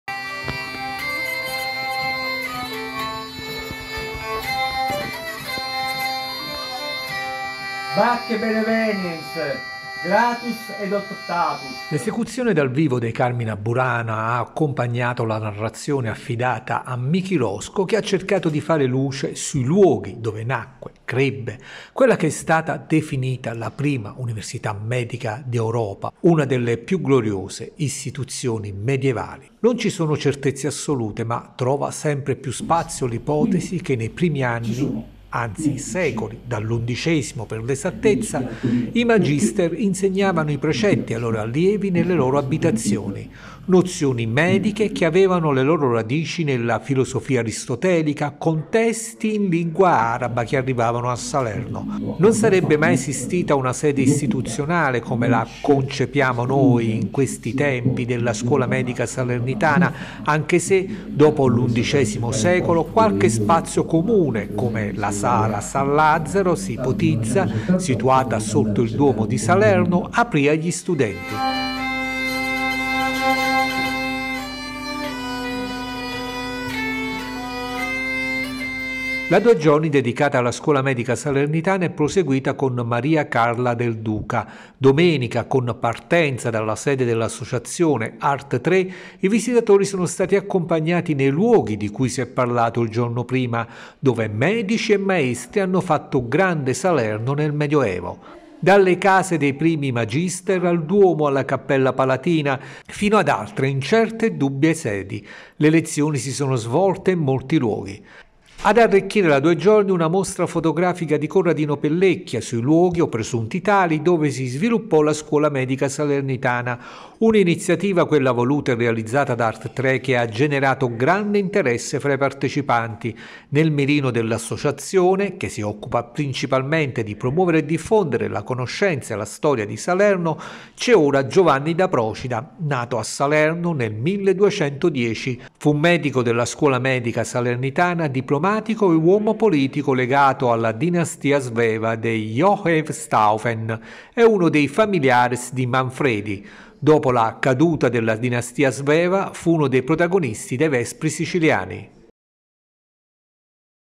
Ancora avvolta nel mistero parte della vicenda della scuola medica salernitana
Ancora avvolta nel mistero parte della vicenda della scuola medica salernitana. I luoghi della gloriosa istituzione sono stati al centro di una due giorni organizzata  da Art.Tre